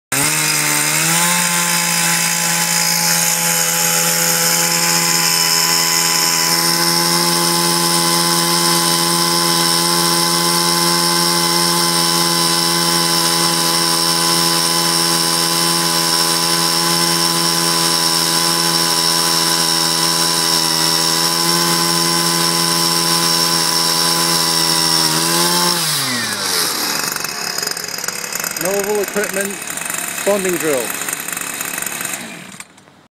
Rail Bonding Drill
Lightweight rail bonding drill in action.